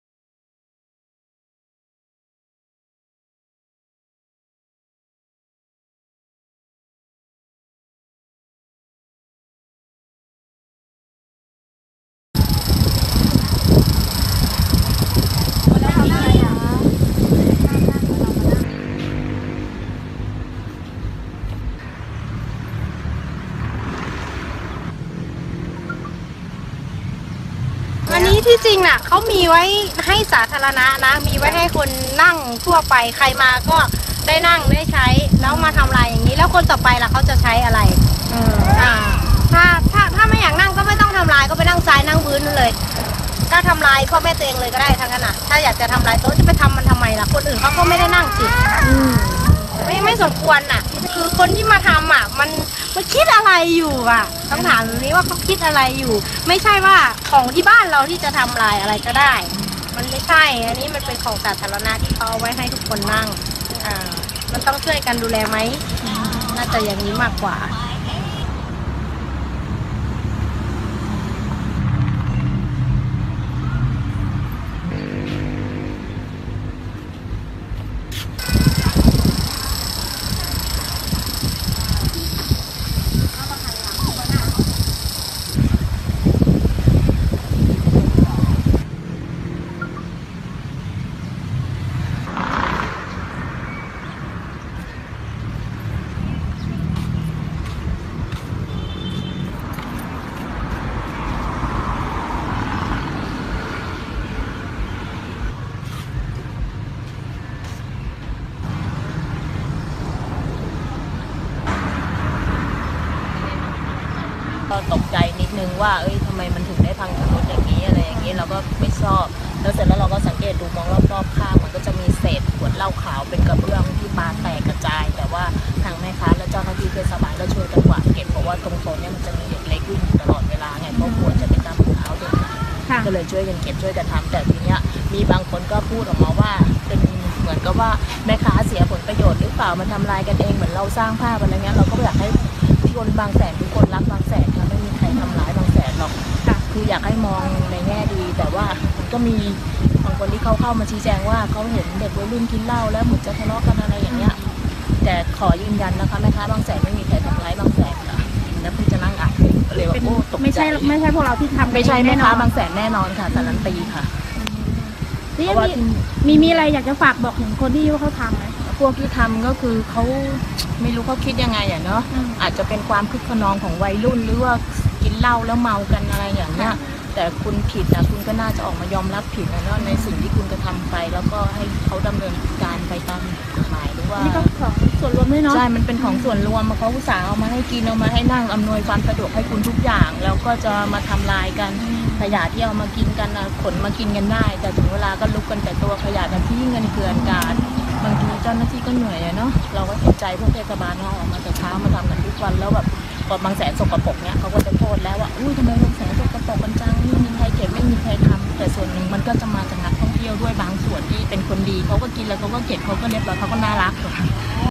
เดือด!นายกตุ้ย โพสต์เฟซบุ๊กติดแฮชแท็กหนักแผ่นดิน ฉะคนทำลายโต๊ะนั่งริมหาดบางแสน
เดือด!นายกตุ้ย โพสต์เฟซบุ๊กติดแฮชแท็กหนักแผ่นดิน ฉะคนทำลายโต๊ะนั่งริมหาดบางแสน 13/01/2020